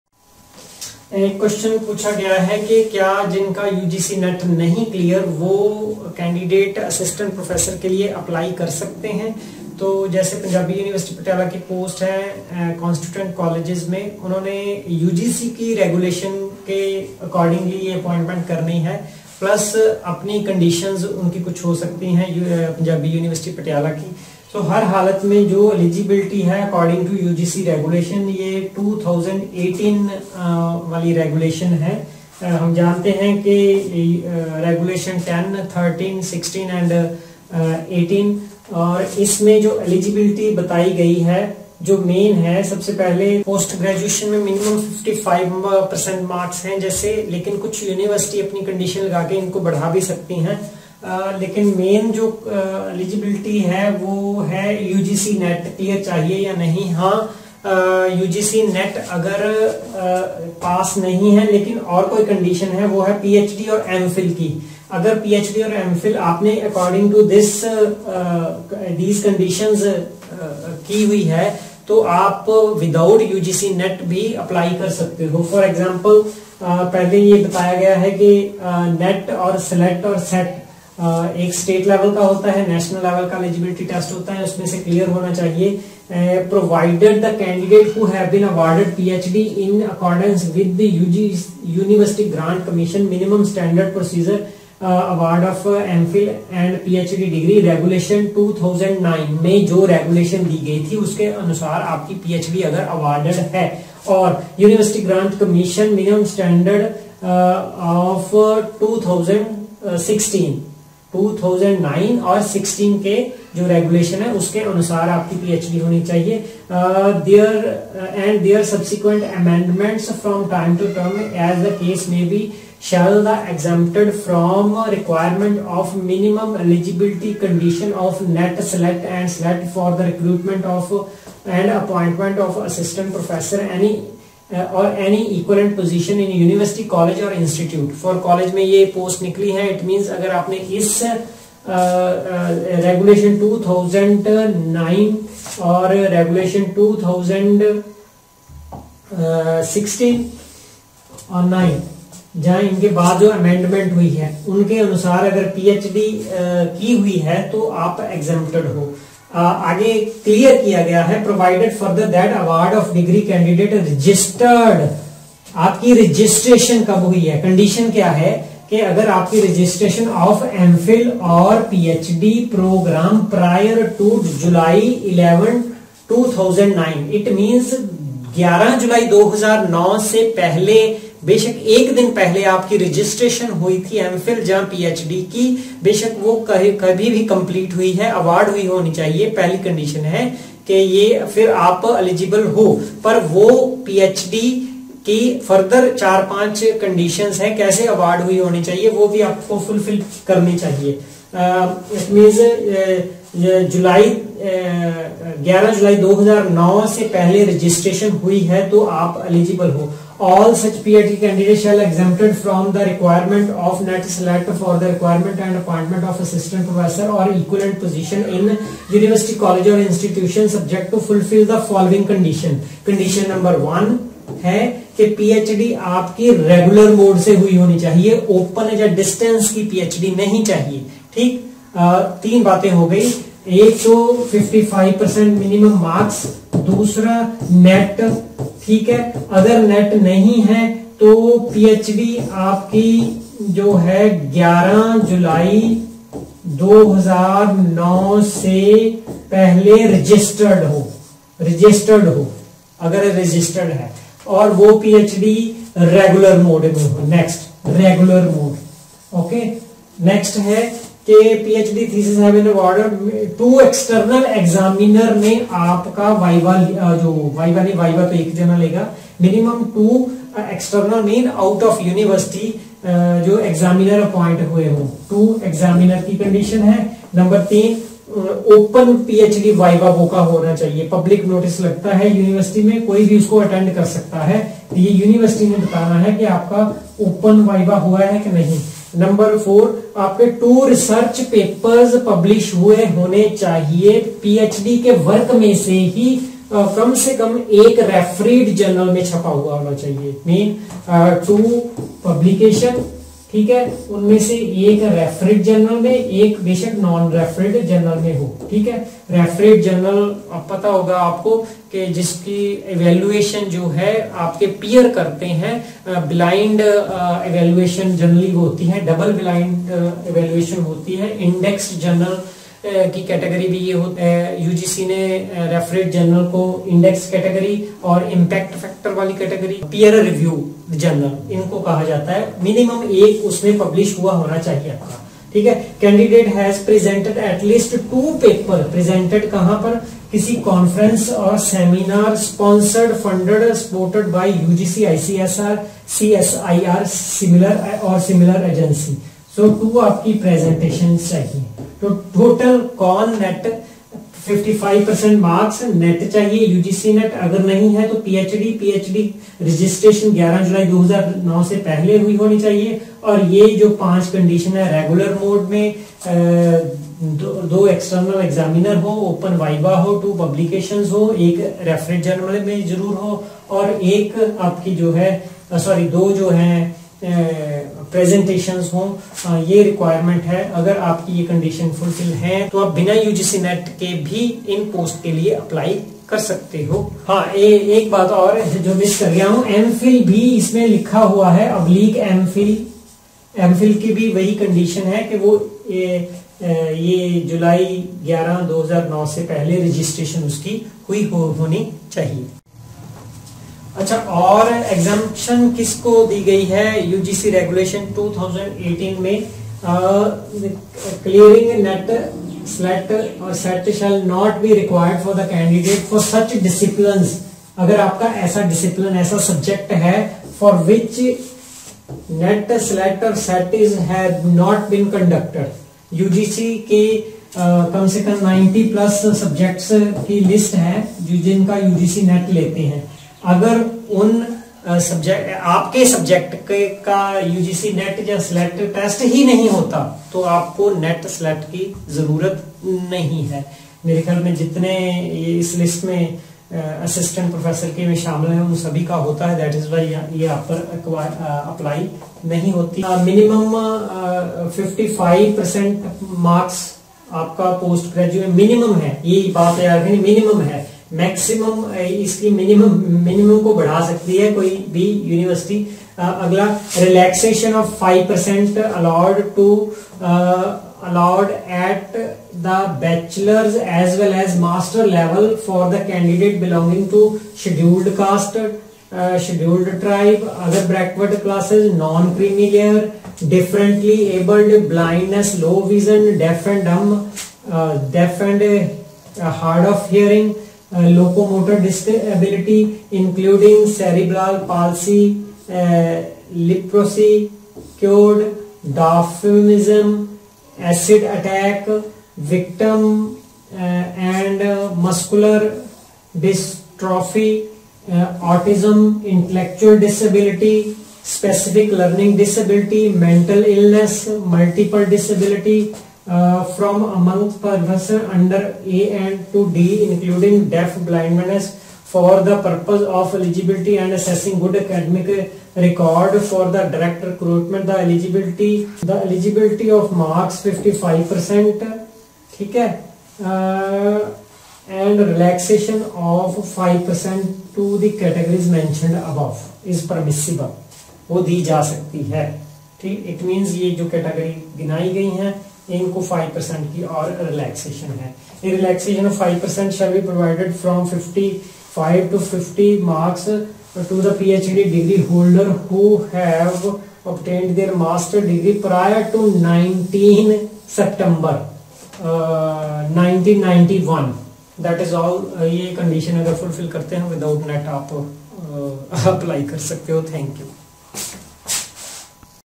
Eligibility for Assistant Professor l UGC Regulation 2018 l Dr. Tirath Singh l Learn to Actualize
Exemptions from UGC NET